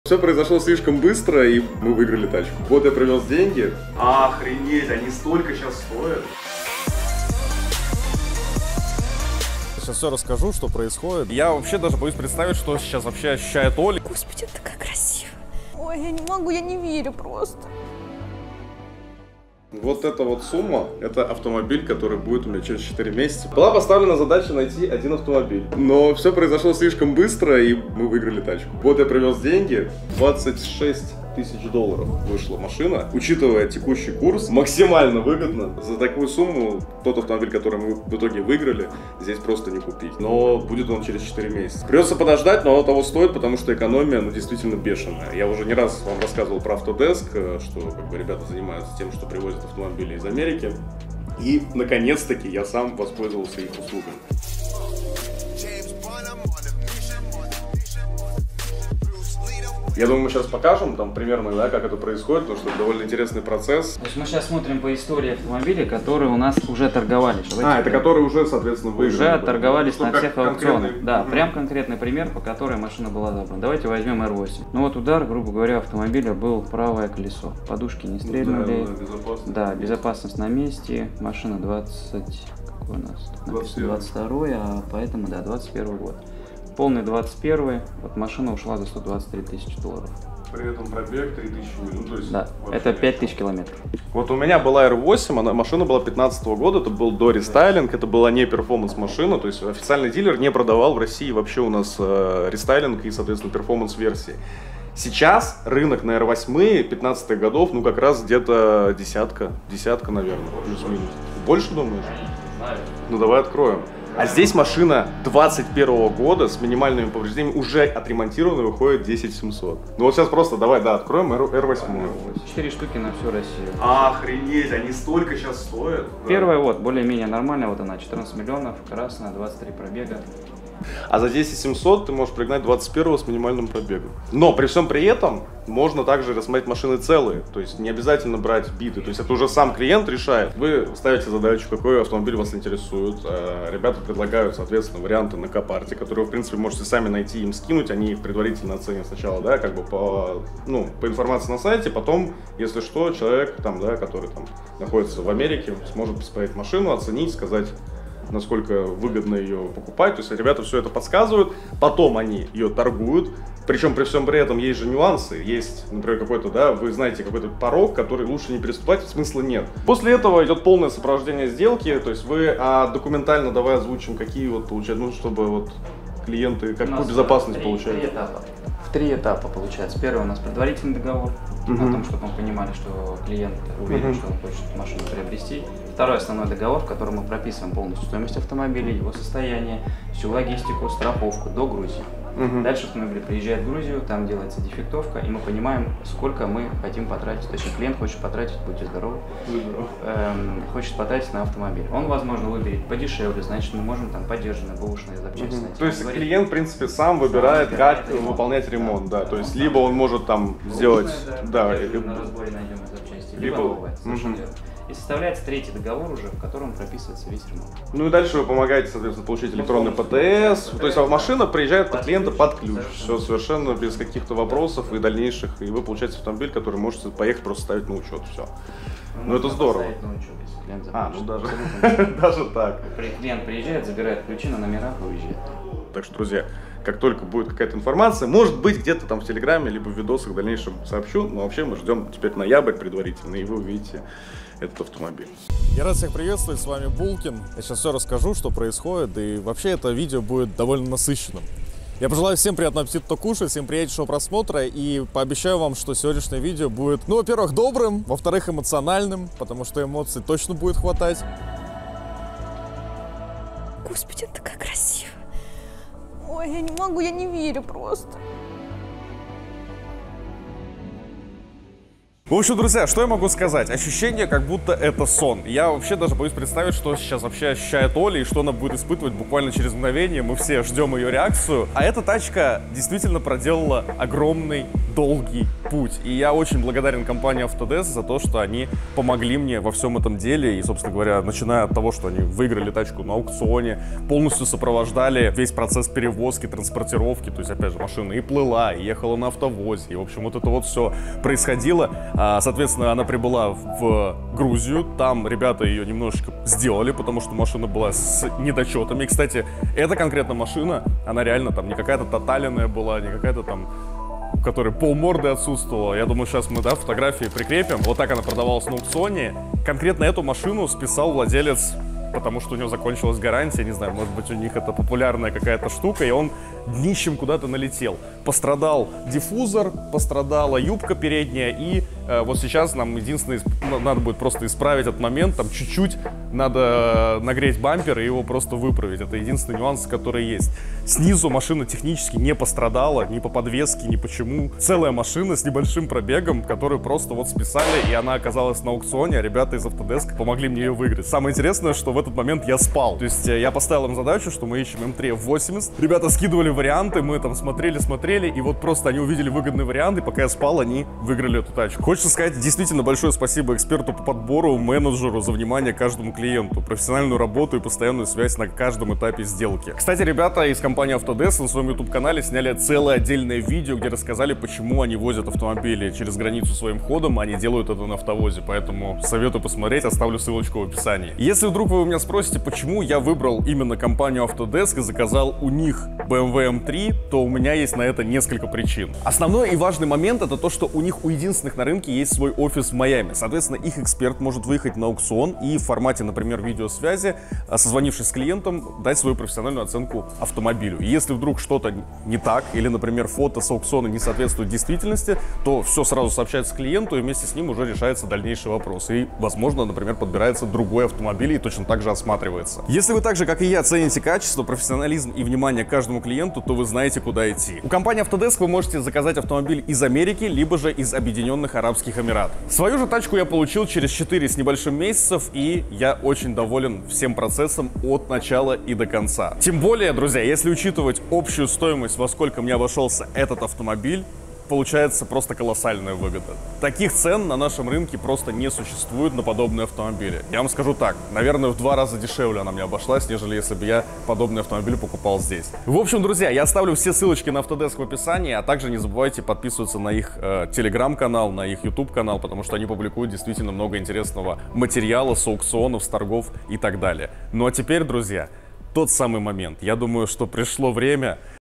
Доставили M3 нашему клиенту.
Мы были рады принять участие в подготовке и проведении покупки и доставки автомобиля нашему клиенту и популярному блогеру Александру Булкину! Автомобиль стал подарком для его жены Ольги и это самое приятное событие для нас, когда мы становимся частью чего-то большего и радуем не только наших клиентов, но и их родных и близких.

Если Вы также хотите заказать автомобиль из США, Европы или ОАЭ, переходите в наш ТГ канал и оставляйте заявку нашим менеджерам.